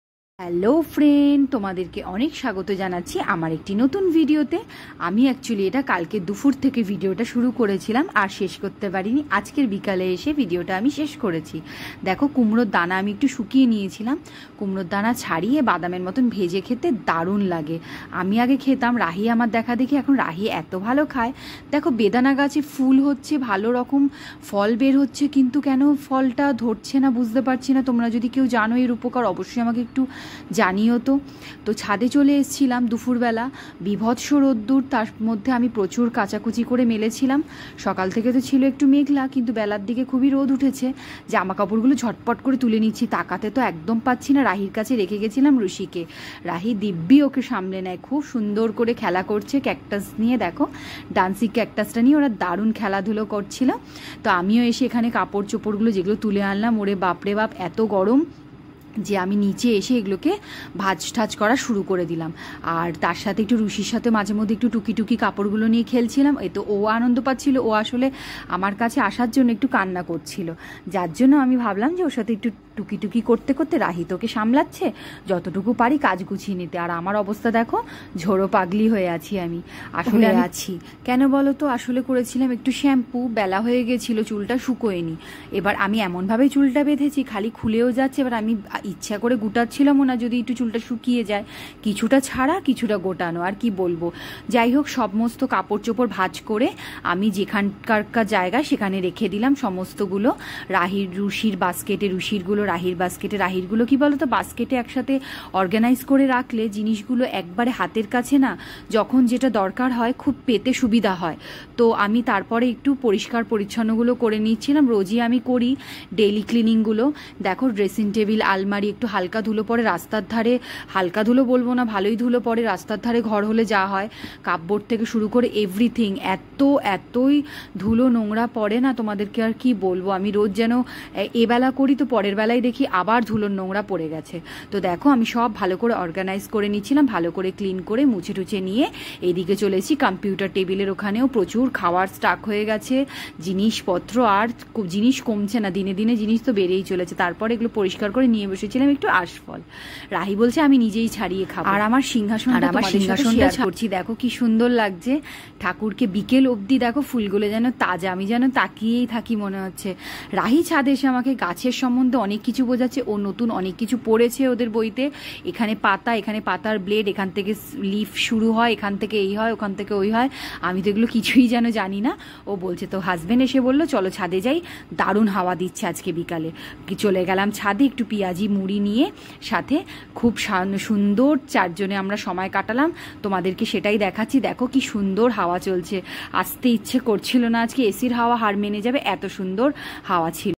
মেয়ের পড়াশোনা, ঘরের কাজ একা হাতে কি ভাবে সব সামলাই 🙏ভালো লাগলে subscribe করে পাশে থাকবেন 🙏❤️
মেয়ের পড়াশোনা, ঘরের কাজ একা হাতে কি ভাবে সব সামলাই 🙏